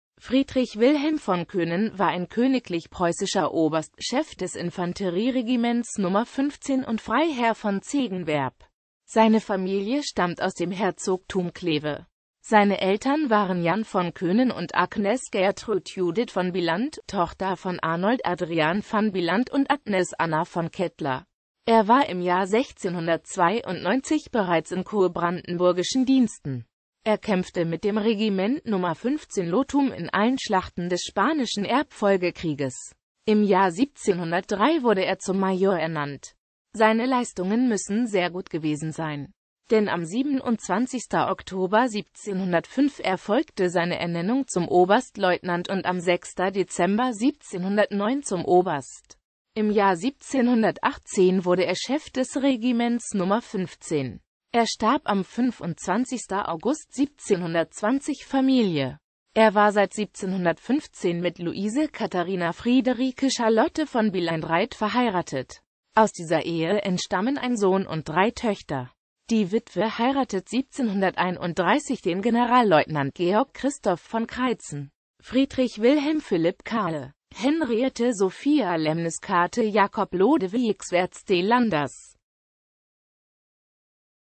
Friedrich Wilhelm von Koenen
Vielen Dank für Ihre Unterstützung:

Friedrich Wilhelm von Koenen war ein königlich preußischer Oberst, Chef des Infanterie-Regiments Nummer 15 und Freiherr von Zegenwerp .Seine Familie stammt aus dem Herzogtum Kleve.Seine Eltern waren Jan von Koenen und Agnes Gertrud Judith von Bylandt, Tochter von Arnold Adriaan van Bylandt und Agnes Anna von Kettler .Er war im Jahr 1692 bereits in kurbrandenburgischen Diensten.